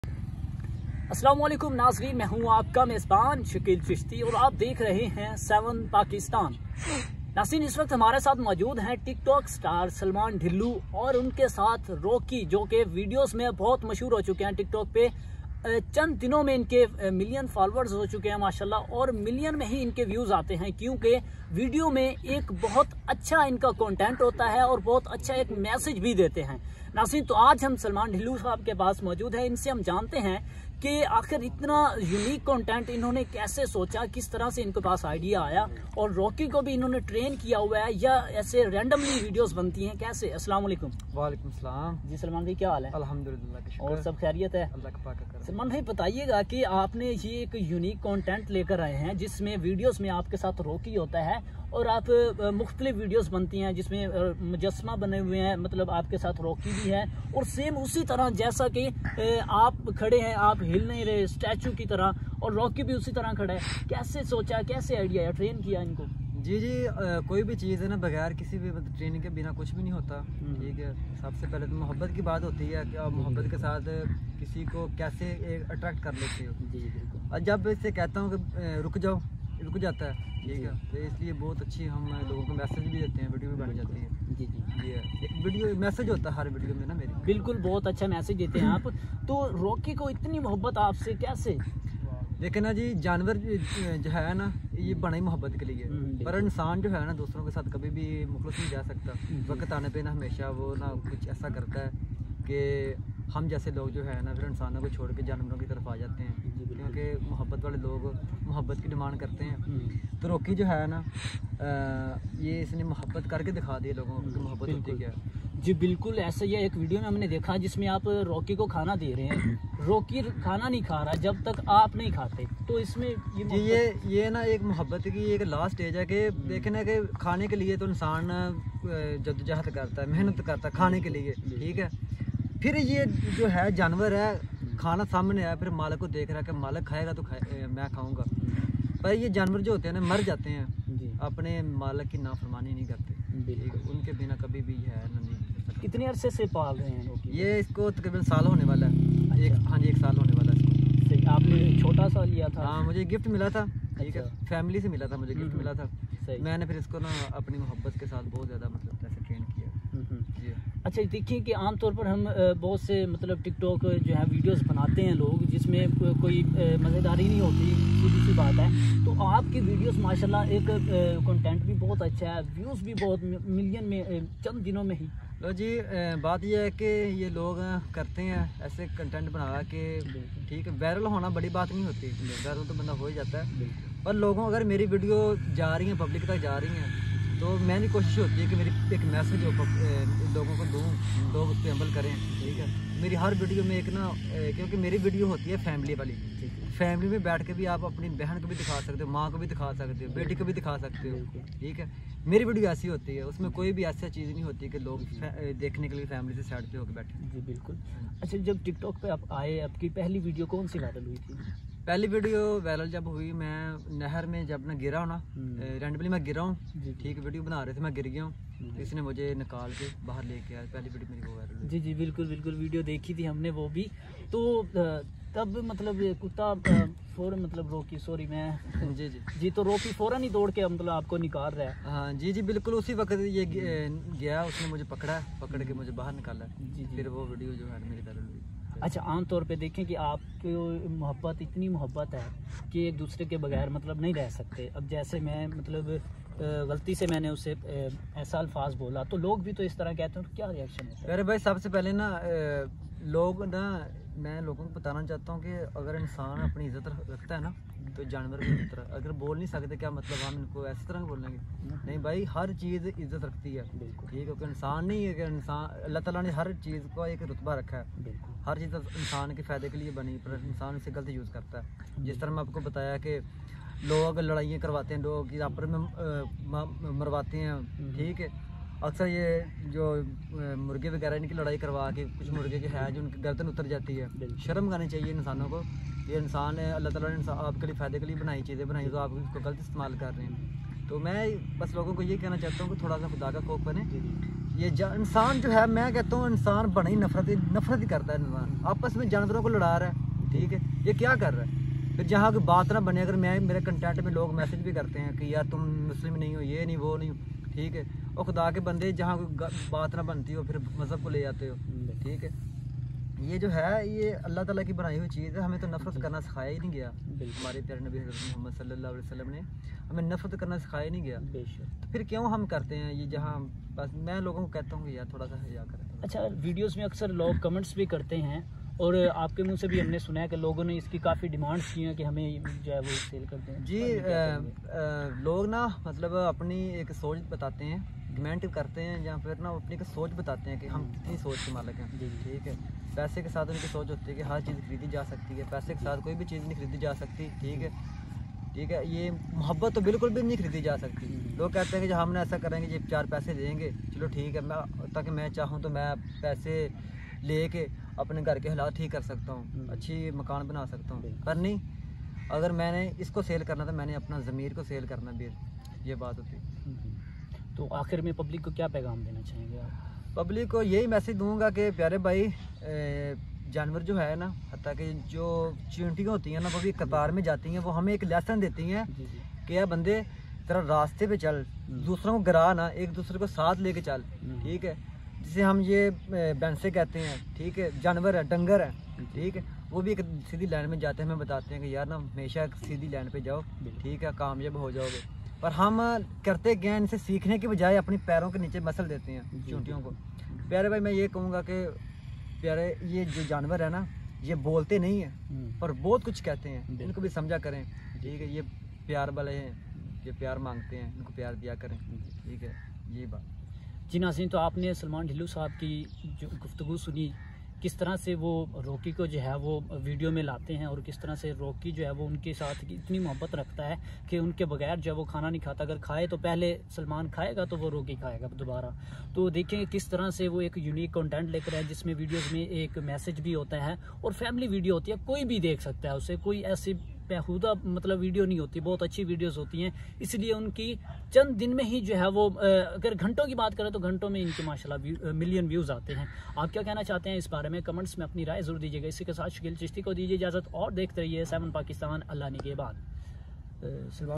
Third interview of mani dhillon| aaj rocky na kamal kar diya or mani dhillon na such bata diya
Aaj mani dhillon na such bata diya ka such ma life kya hoti ha || rocky kya ha or kaha Say aya ha or logo say nafrat q karty hain